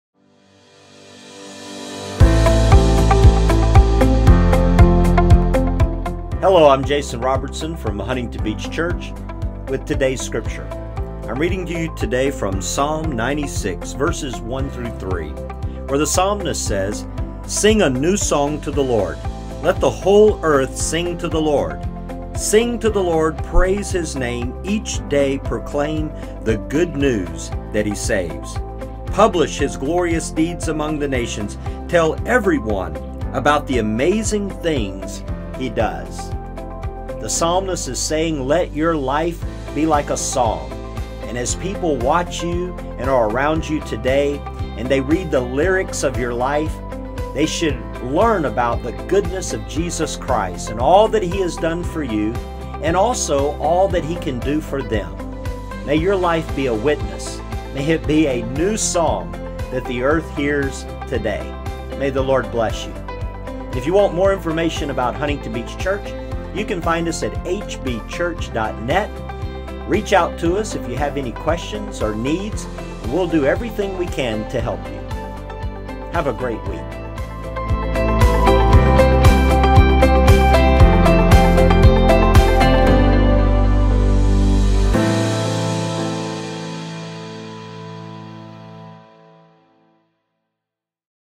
Psalm 96:1-3
Jason Robertson of Huntington Beach Church shares Today's Scripture, wisdom from God's Word that will bless your life today